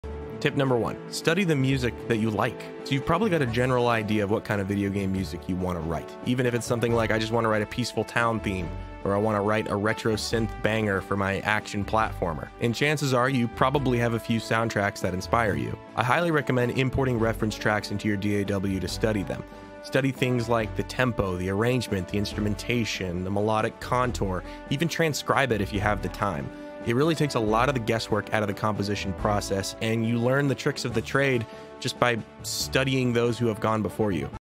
Want to write video game music? Do this.
This is one of the best ways to get better at writing VGM, and it costs literally zero money.